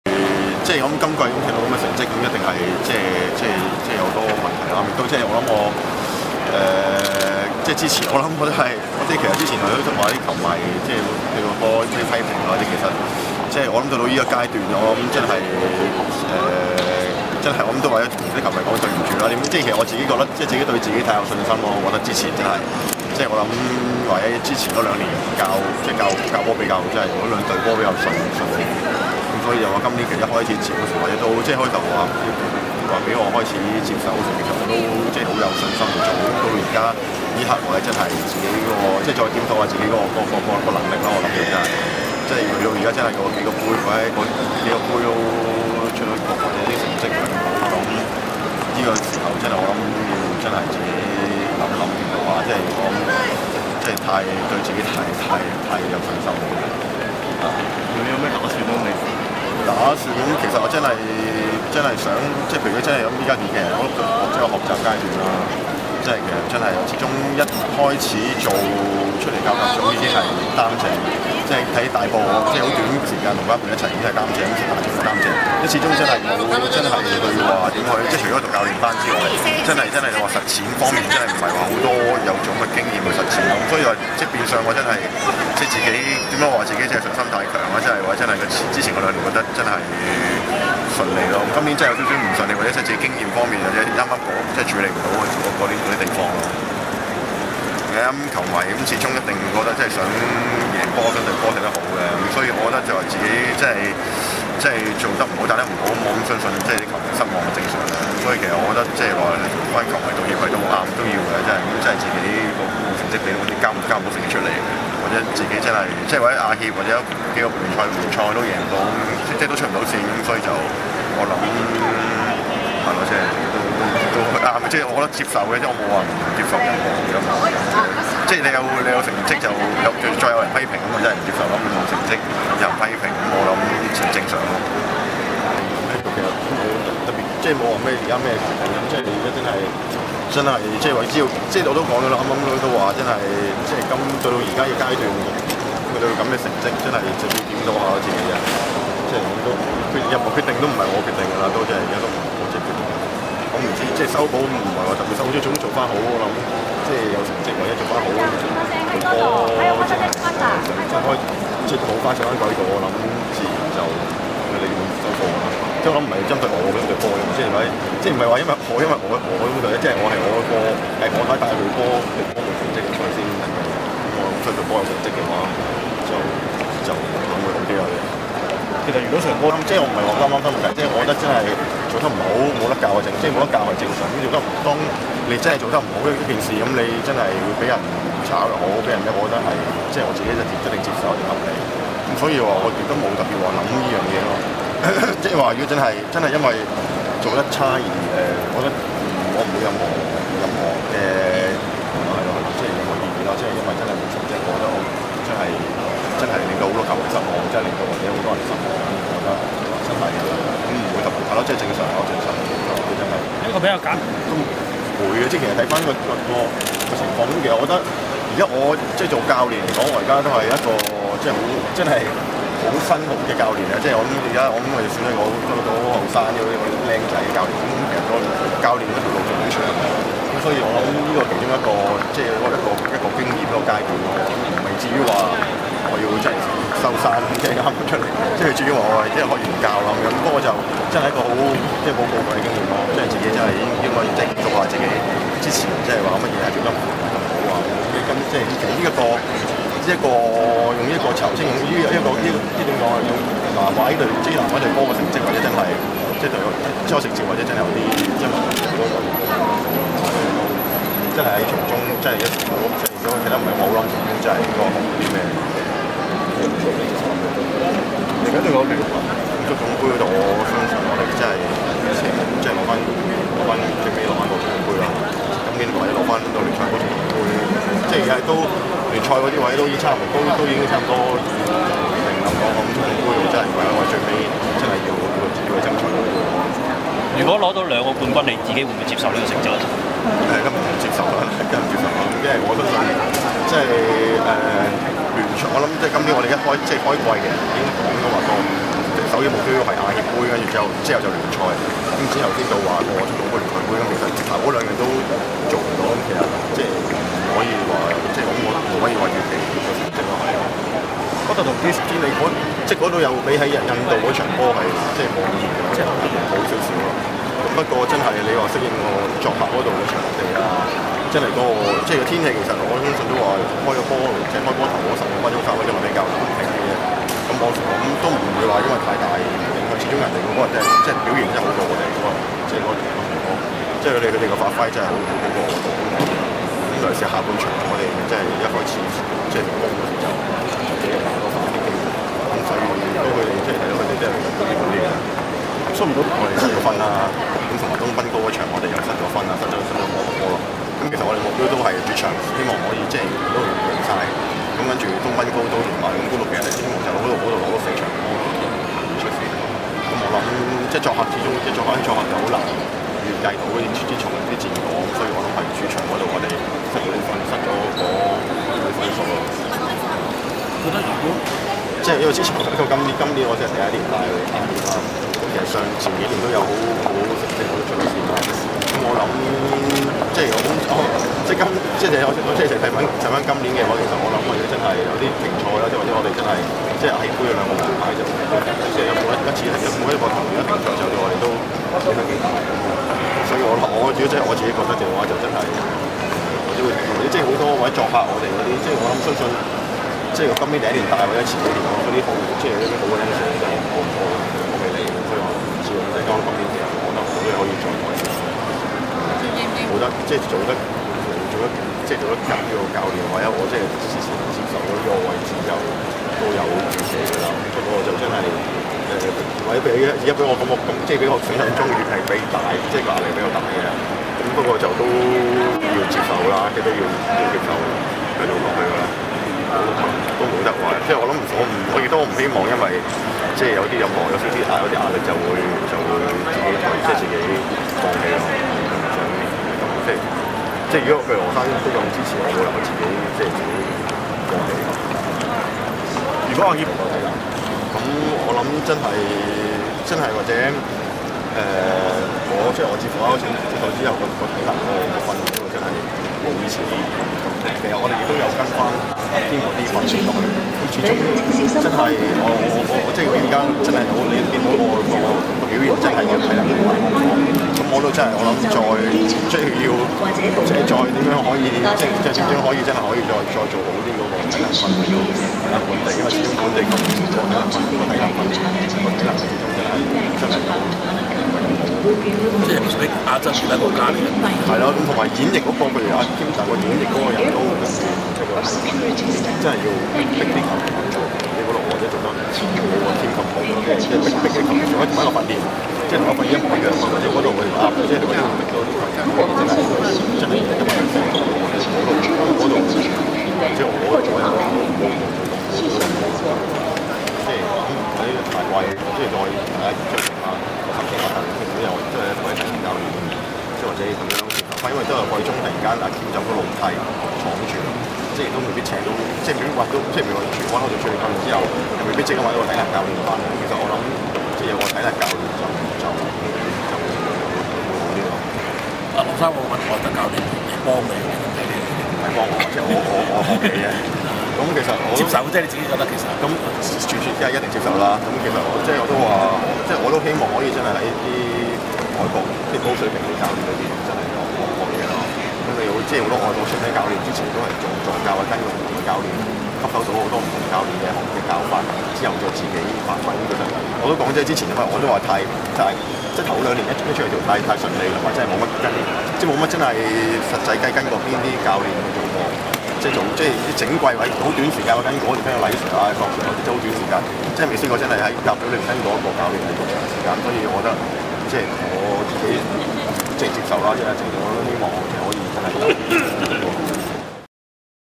南華抵港，陳浩然見記者(by keymansoho)
2011年5月4日

南華於亞協出局，大軍於賽後返抵香港。署理教練陳浩然就成績欠佳而向球迷道歉。